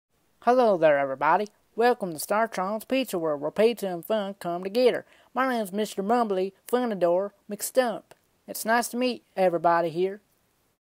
Mumbly mcstump voice preview
This vocal preview took 11 takes/redos. The voice is not final and may be changed.